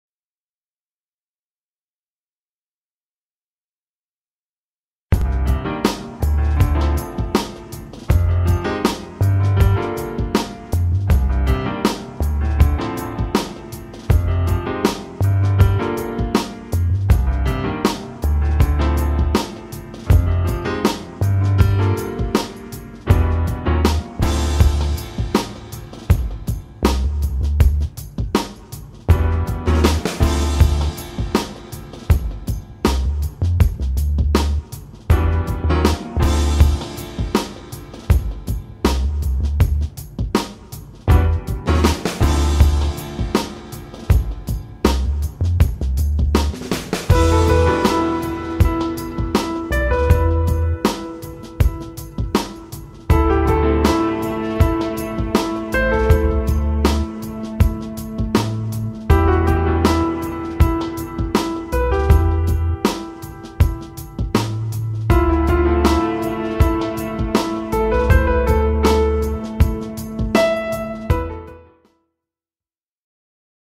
Volkswagen tiguan occasion visible à La teste-de-buch présentée par Auto 33
Cette Volkswagen tiguan occasion disponible à La teste-de-buch dans le 33 est mise en vente par Auto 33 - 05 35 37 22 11 - tous les détails sont